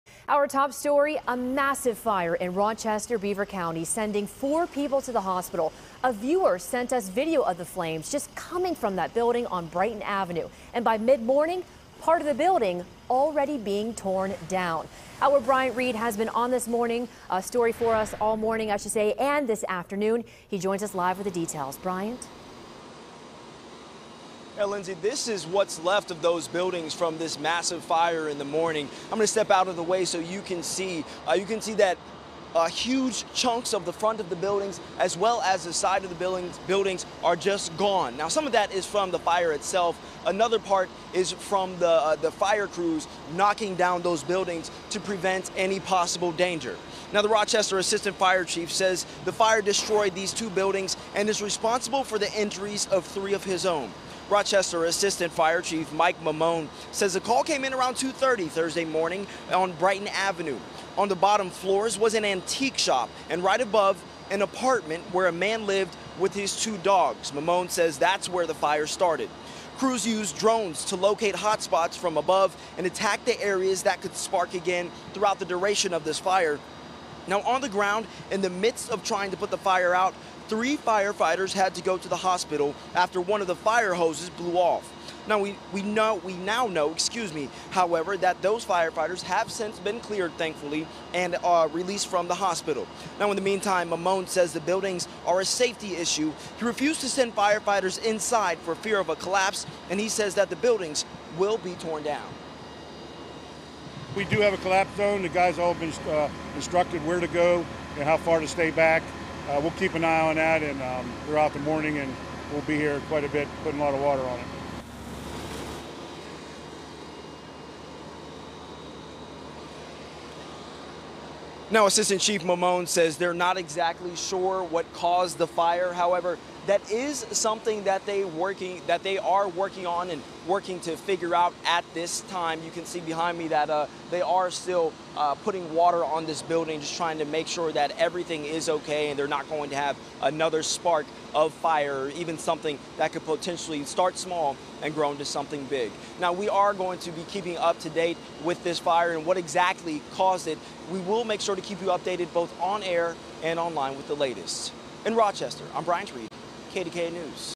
Crews working to demolish building after overnight fire in Beaver County
Crews are working to demolish a building in Rochester after it went up in flames overnight; KDKA's Bryant Reed reports.